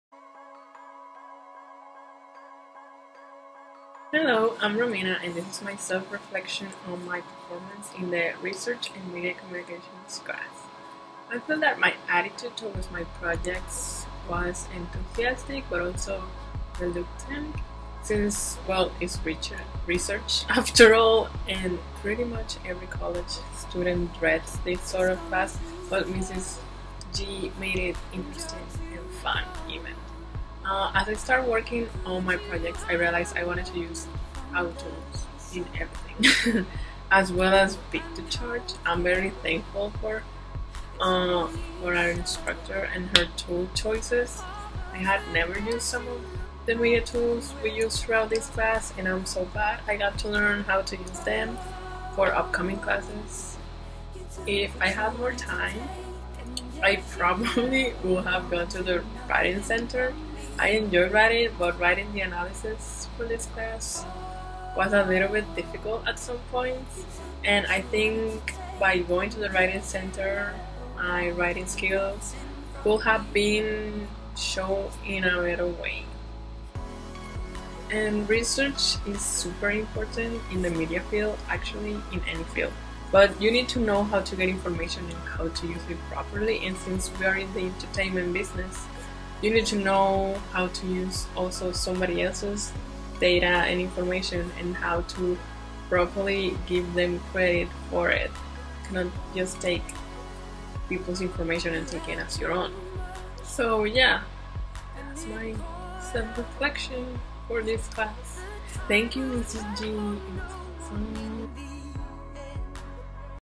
Self - Reflection RMC
Using the four tiers of the RISE Model as a guide, perform a self-evaluation on the projects you completed this month.